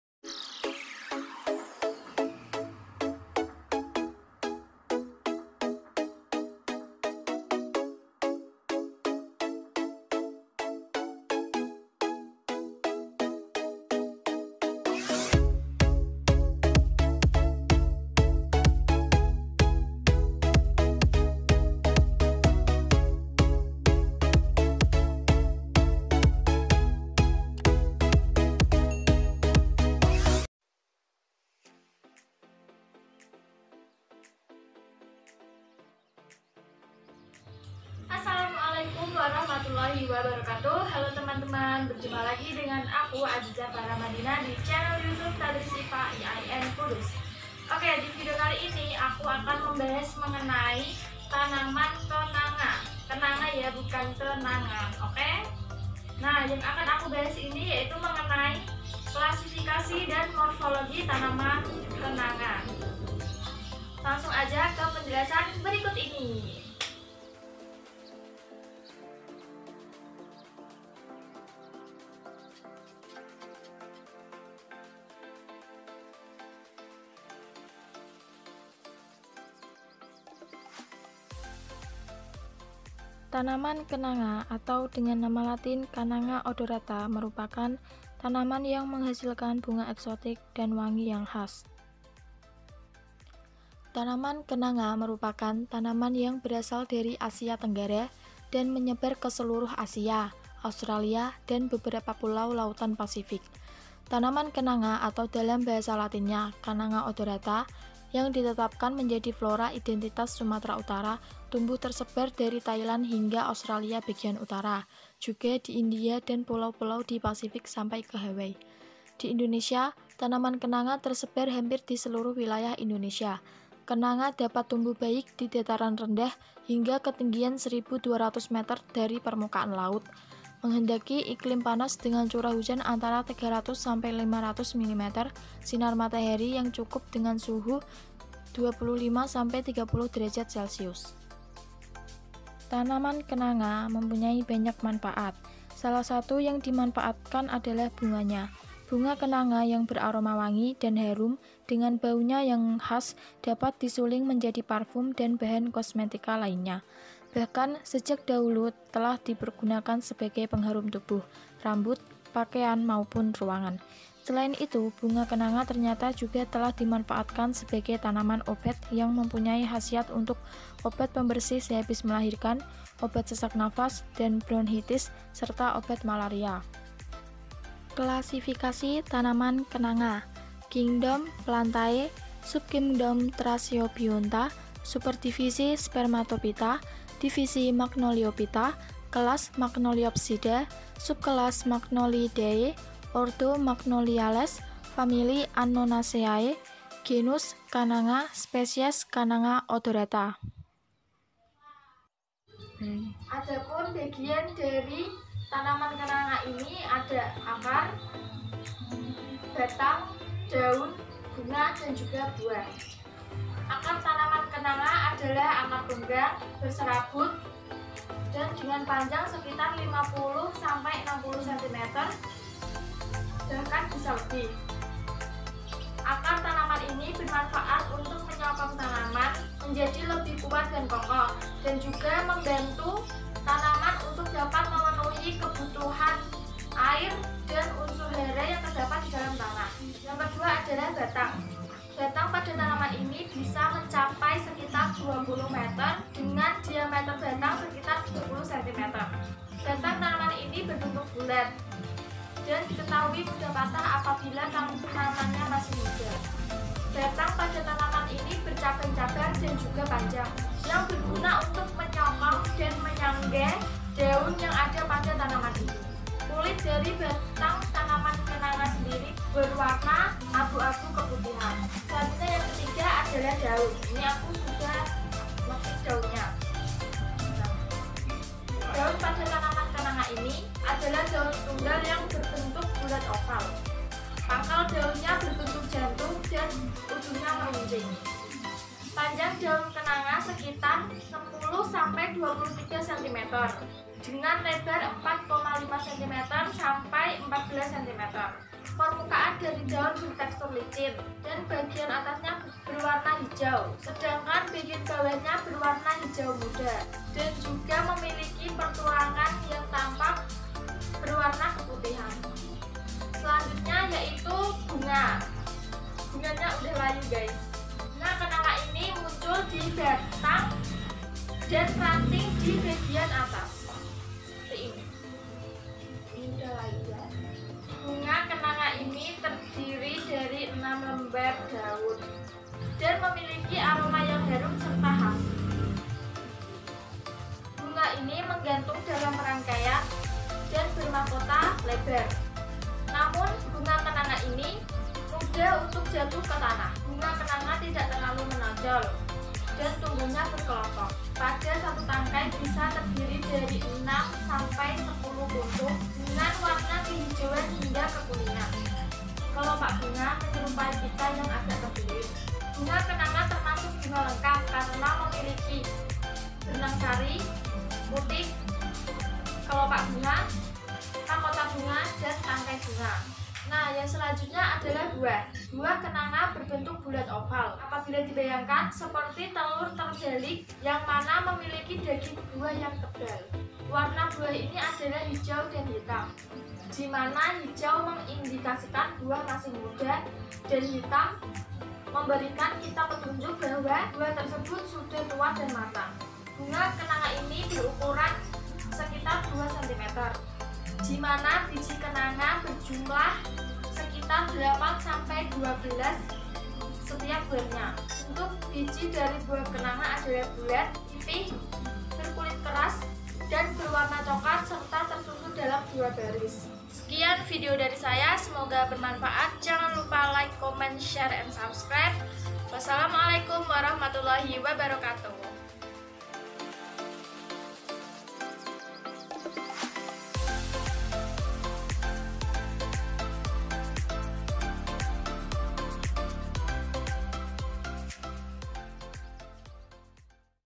Klasifikasi dan Morfologi Tanaman Kenanga |KKN-IK-DR IAIN KUDUS| TADRIS IPA| Kelompok 82
Vlogger : Azizah Paramadina
DPL : Ulya Fawaida, M.Pd

Vlog ini dapat digunakan sebagai sumber belajar pada SMP kelas
3.2 Mengklasifikasikan makhluk hidup dan benda berdasarkan karakteristik yang diamati